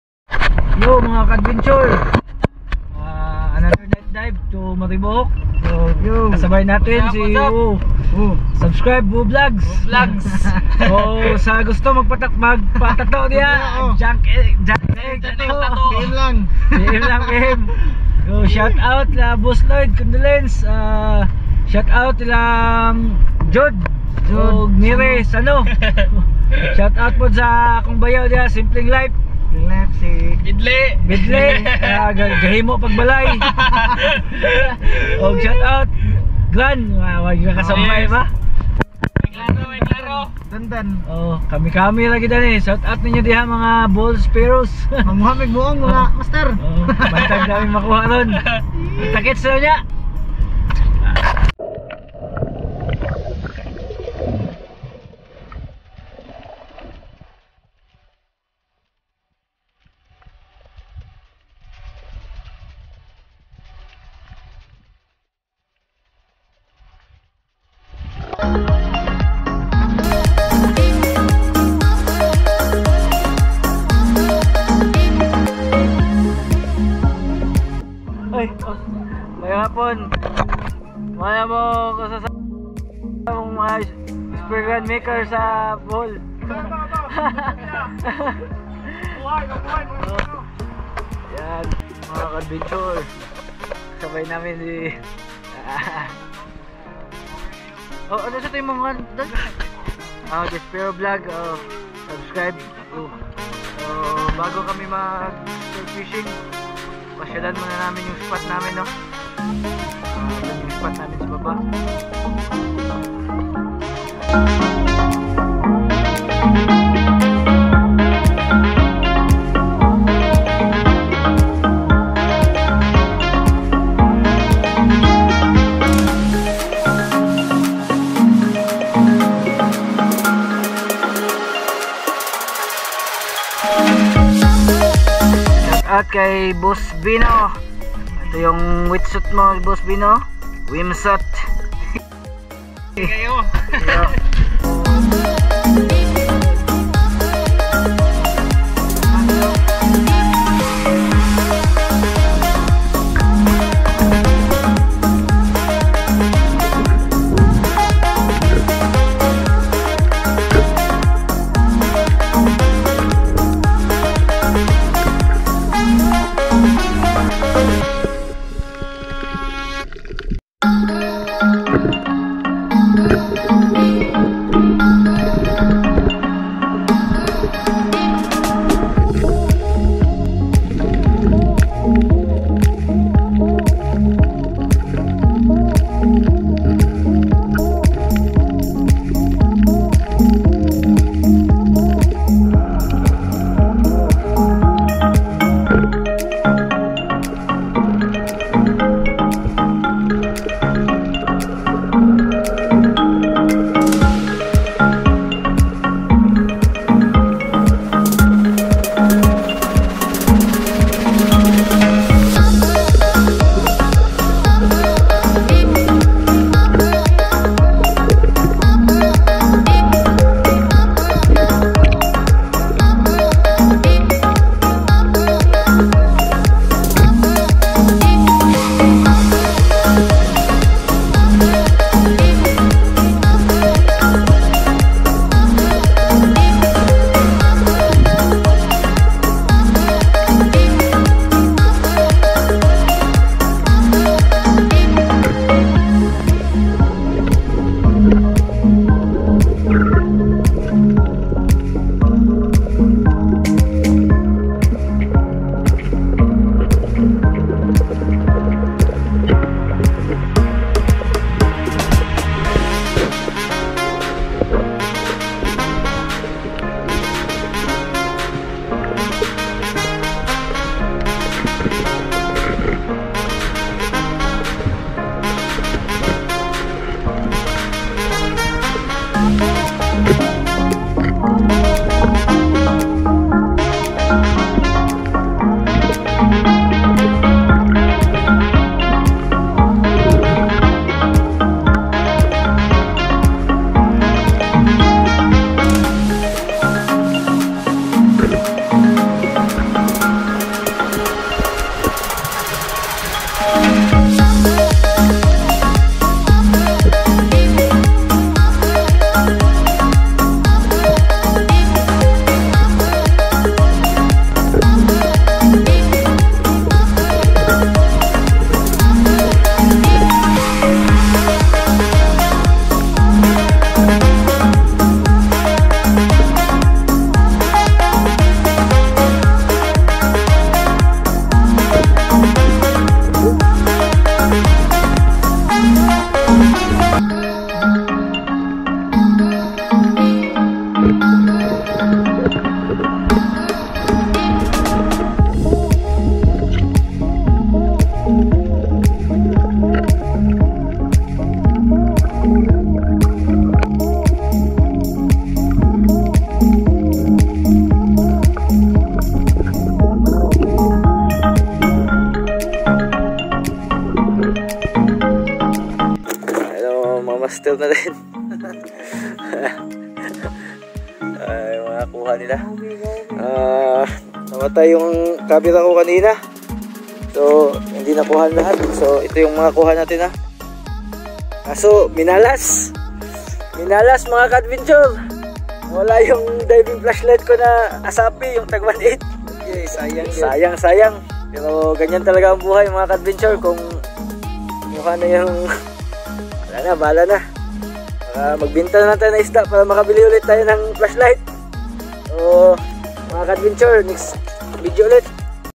Night Spearfishing at Maribojoc Bohol
Minalas lang kasi nawala yung Asafee A26 diving Flashlight ko..
If you like this Video please give it a THUMBS UP 👍.
SALAMAT MGA KA ADVENTURE 🤙 | Lotax Adventure